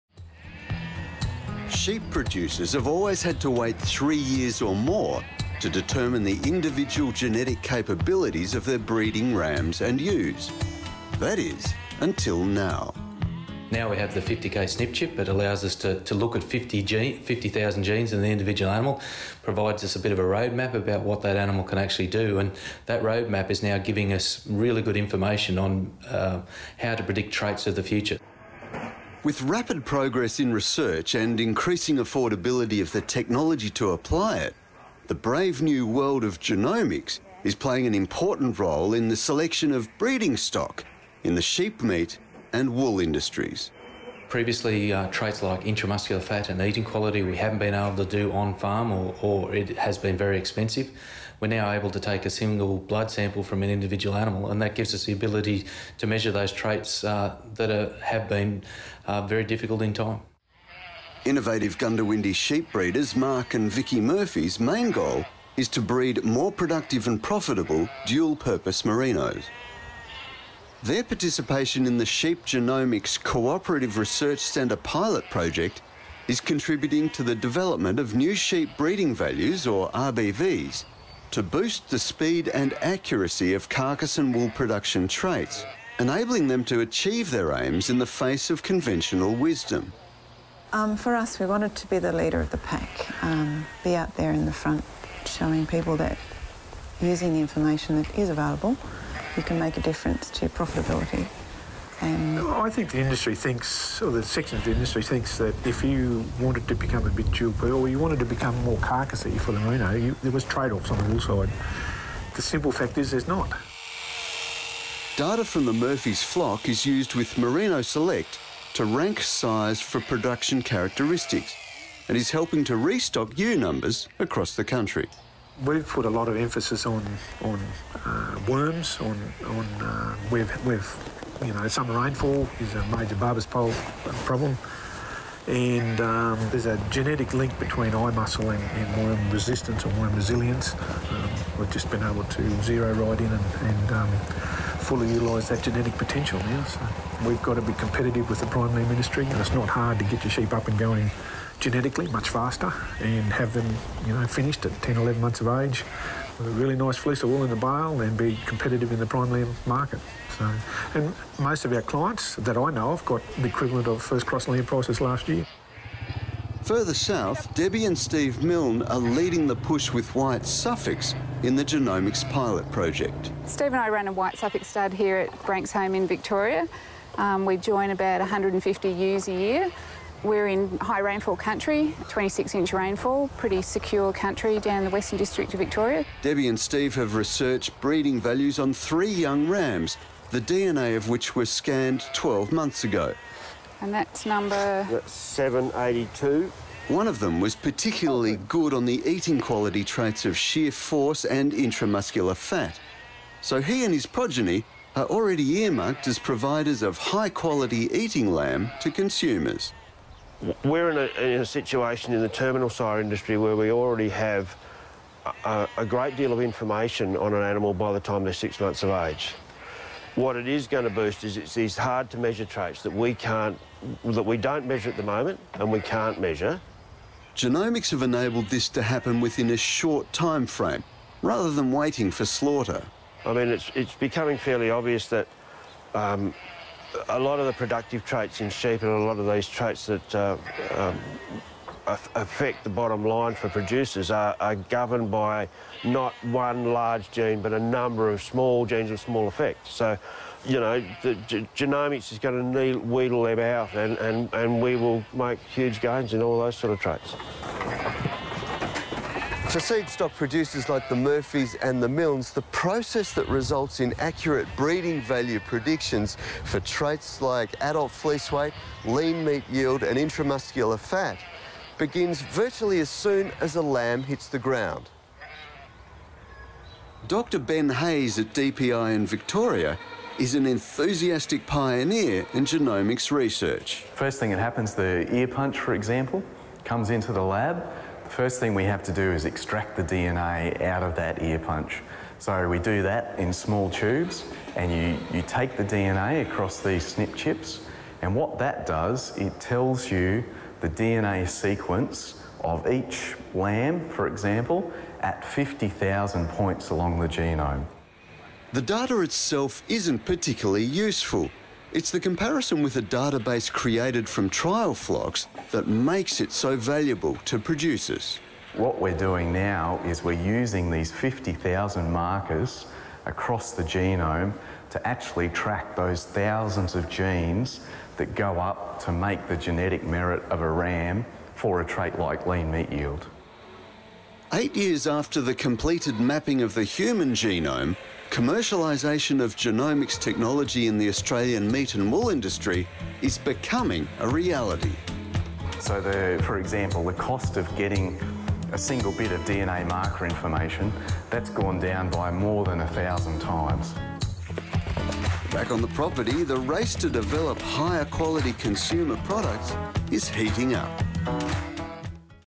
Sheep genomics -- Chip off the old flock (ep 15 feedbackTV)
New discoveries in sheep genomics are leading to the development of new sheep breeding values to boost the speed and accuracy of carcase and wool production traits.